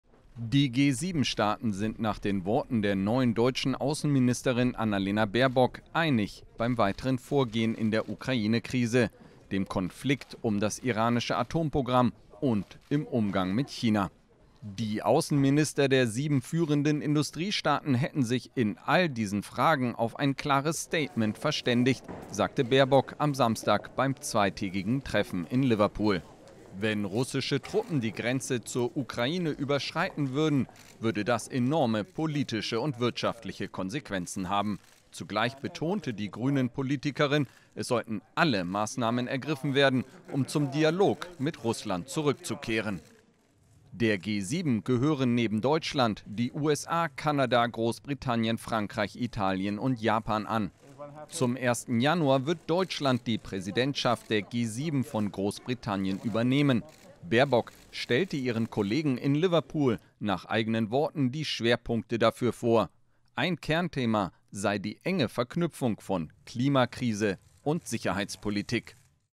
G7 beim Gipfel in Liverpool einig im Vorgehen mit Russland und China
Zum 1. Januar wird Deutschland die Präsidentschaft der G7 von Großbritannien übernehmen.